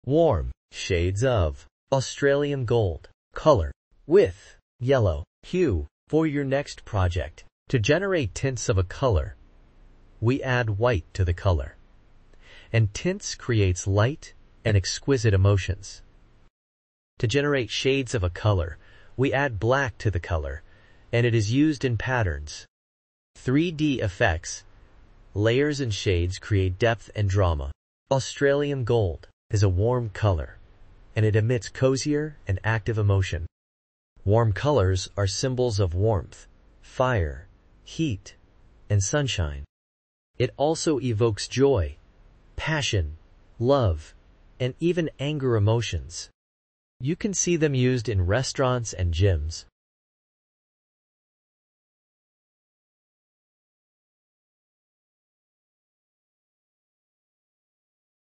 Shades & Tints of Australium Gold color e7b53b A Warm Yellow color
Shades of Australium Gold color e7b53b Warm Shades of Australium Gold color with Yellow hue for your project!

Shades
Hex cda034

Tints
Hex e9bd50

Australium Gold is a warm color, and it emits cozier and active emotion. Warm colors are symbols of warmth, fire, heat, and sunshine. It also evokes joy, passion, love, and even anger emotions. You can see them used in Restaurants and Gyms.